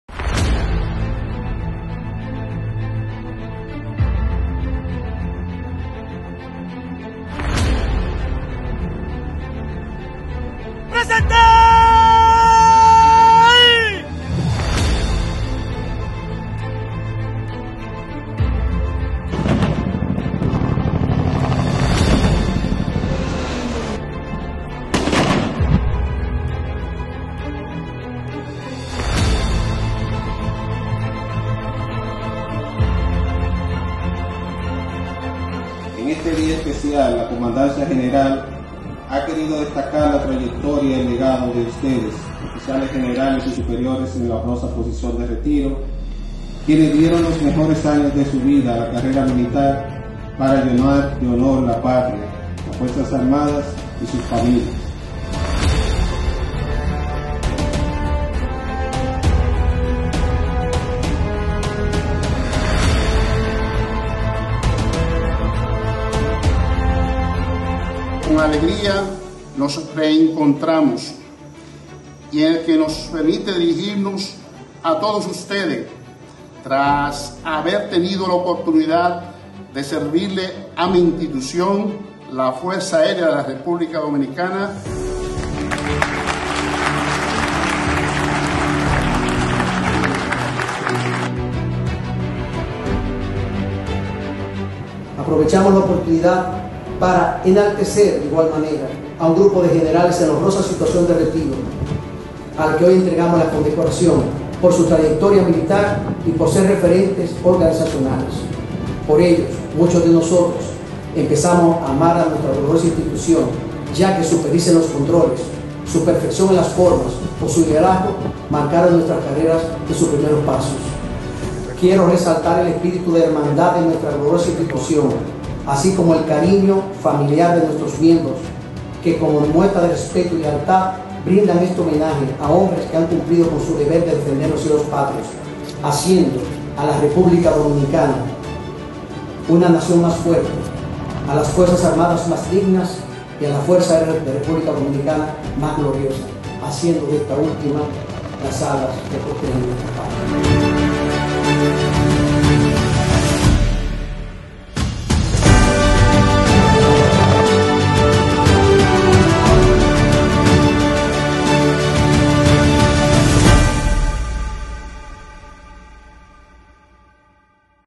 [Ceremonia de despedida A Generales puesto en situación de honroso retiro]
[FARD realiza homenaje a Oficiales Generales puestos en retiro]

La Fuerza Aérea de República Dominicana reconoció a un grupo de oficiales generales y superiores que fueron colocados en la honrosa posición de retiro.

El acto fue encabezado por el Teniente General Carlos Díaz Morfa, ERD, Ministro de Defensa y el Comandante General de esta institución, Mayor General Piloto, Leonel Muñoz Noboa.

La mesa de honor estuvo compuesta por el Mayor General Piloto Juan Jiménez García, Viceministro de Defensa para Asuntos Aéreos y Espaciales; Mayor General Julio Florián Pérez, Comandante General, ERD; Vicealmirante Ramón Betances Hernández, Comandante General, ARD; Mayor General Edward Sánchez González, Director General de la Policía Nacional; General de Brigada Técnico de Aviación, Carlos Febrillet Rodríguez, Director del CESAC; General de Brigada Piloto Moltimer De Jesús Eusebio, Subcomandante General FARD; General de Brigada Piloto, Francisco Corominas Sánchez, Director de Inteligencia A-2; el Coronel Piloto, Rafael Acosta Sena, Comandante Base Aérea San Isidro y el Coronel Piloto, Manuel García Lithgow, Comandante Base Aérea de Puerto Plata.

El Mayor General Muñoz condecoró al Ministro de Defensa con la Medalla “Vuelo Panamericano”, reconocimiento que por primera vez se otorga en esta institución. Este hecho representa un acontecimiento trascendental para los soldados del aire, ya que se reconoce el esfuerzo, constancia en el cumplimiento de las misiones y méritos logrados por estos.

El Coronel Acosta Sena solicitó el permiso para realizar este emotivo evento, mientras que el General de Brigada De Jesús Eusebio realizó el discurso de bienvenida a todos los presentes.

Los Oficiales Generales que se colocaron en la situación de retiro por medio al Decreto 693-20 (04/12/2020) del Poder Ejecutivo fueron: los Mayores Generales (r), Gilberto Delgado Valdez, Julio Souffront Velázquez, Pedro Valenzuela Quiroz, Miguel Paulino Espinal, Ulises Cedano Santana, Oscar Ceballos Hidalgo, Luis Peña Jiménez, Carlos Jiménez Miranda; Generales de Brigada Mortimer Echavarria, Héctor Ureña Rojas y Coronel Ubaldo Veras y Veras, todos de la Fuerza Aérea de República Dominicana.

El Mayor General (r) Delgado dio las palabras de agradecimiento en nombre de los retirados, resaltando lo noble que fue haber servido a la patria y exhortó a los que continúan con ese importante compromiso a dar lo mejor de ellos por su país y su institución.

Del mismo modo, el Comandante General FARD, manifestó sentirse honrado en despedir de forma honrosa a un grupo de hombres del aire que dieron los mejores años de sus vida por la defensa de la nación y que la institución siempre les agradecerá por haber puesto su nombre en alto.